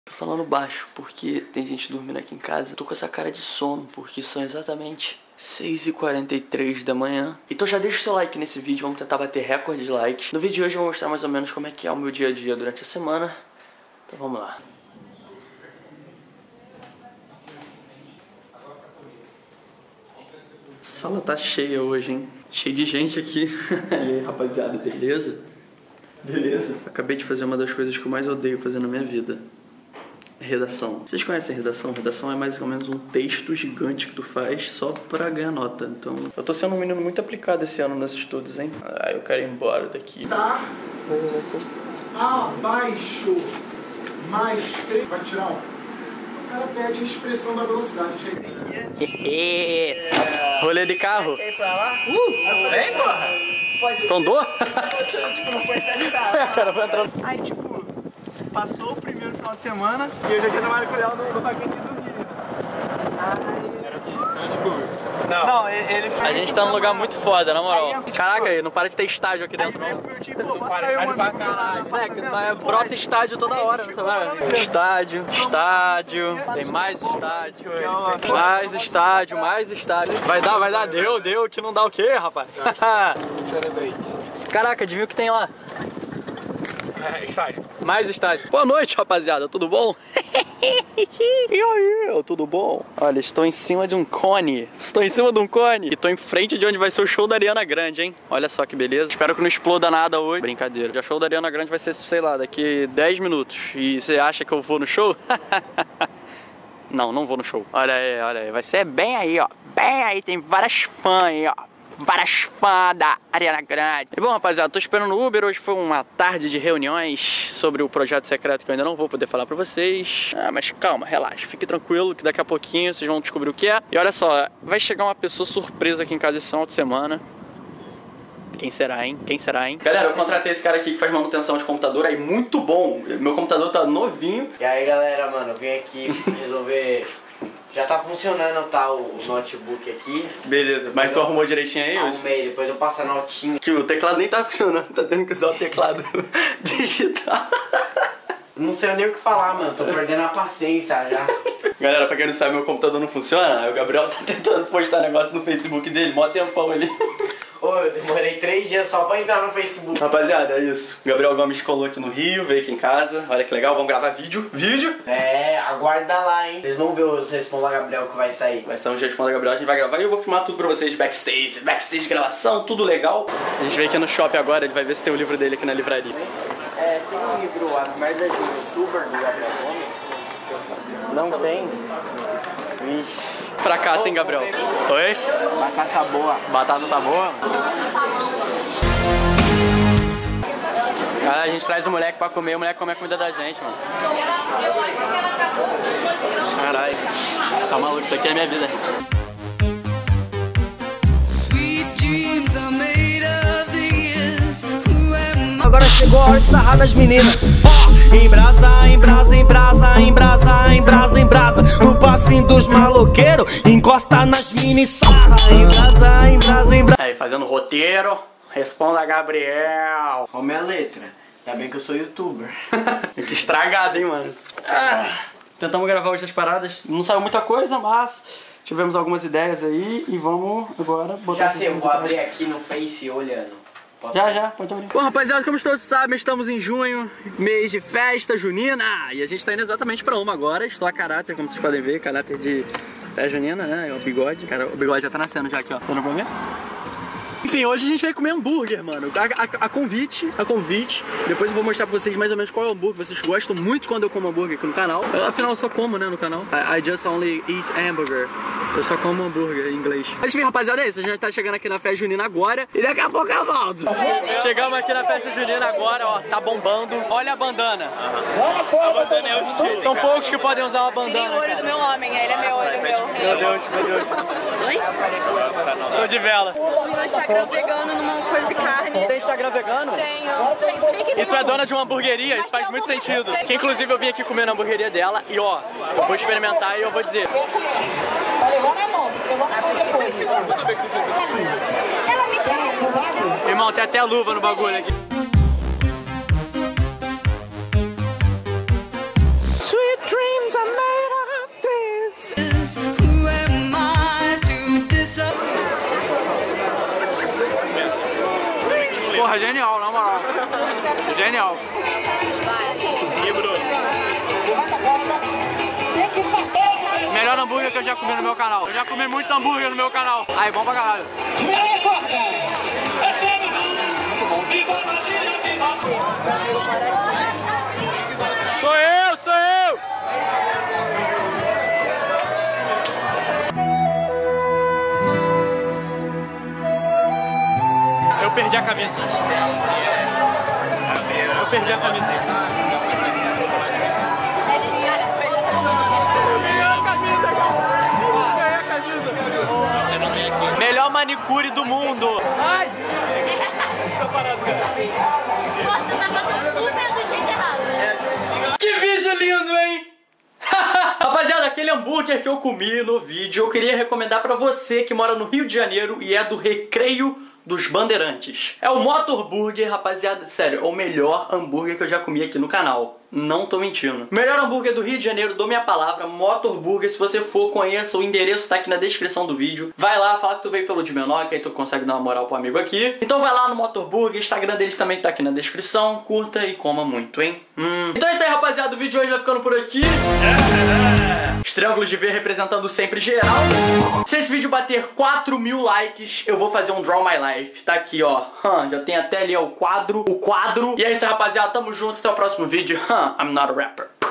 O MELHOR SANDUÍCHE DO MUNDO (Part. Gabriel Gomes)
-Role com Gabriel Gomes-
Motor Burger: Rua Carlos Galhardo 85, Recreio dos Bandeirantes, Rio de Janeiro (RJ)